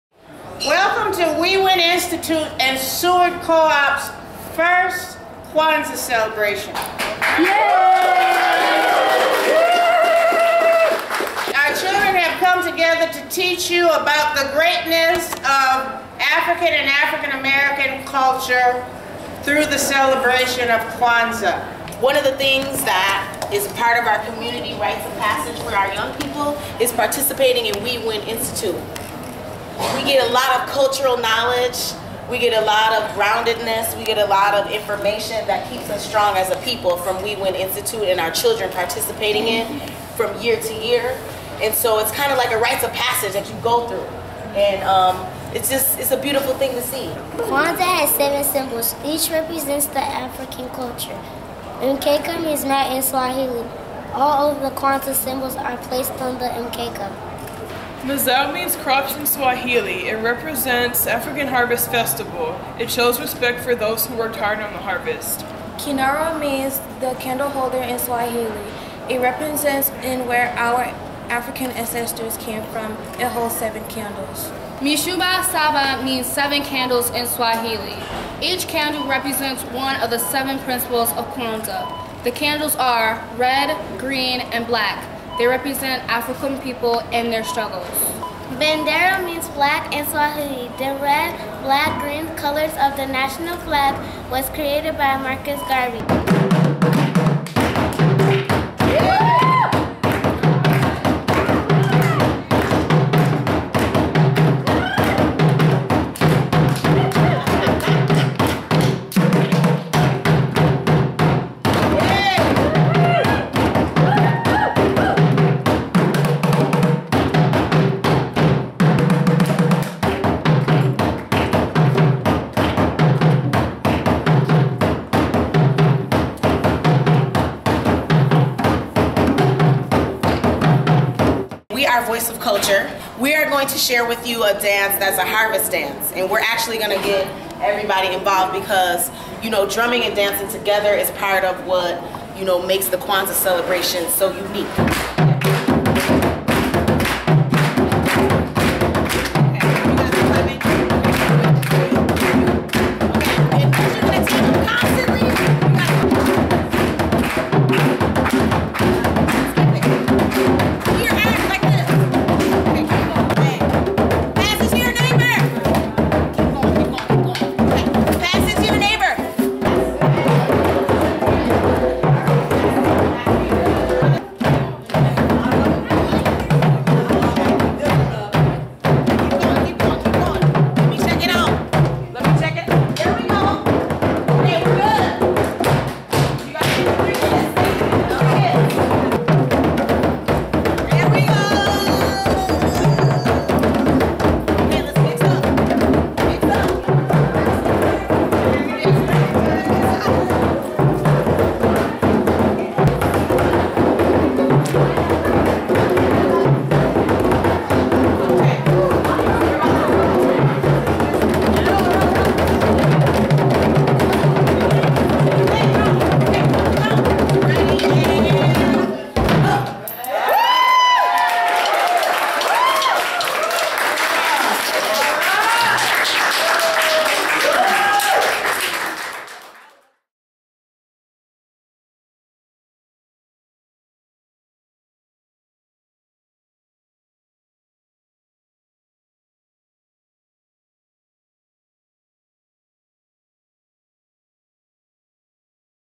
WE WIN Institute's 2015 Kwanzaa Celebration at Seward Friendship Store {12.12.15}
WE WIN Institute teamed up with Nothando Zulu and Kenna Cottman's Voice of Culture to bring 'Kwanzaa Table', a Kwanzaa Celebration to Seward Co-op's Friendship Store.

This event took place in south Minneapolis on December 12, 2015 amongst dozens of community members packed into the hallway of the store.

Pamoja Tutashinda soup was served to the youth and community members at the conclusion of the program.

This video is a quick highlight of the hour plus performance.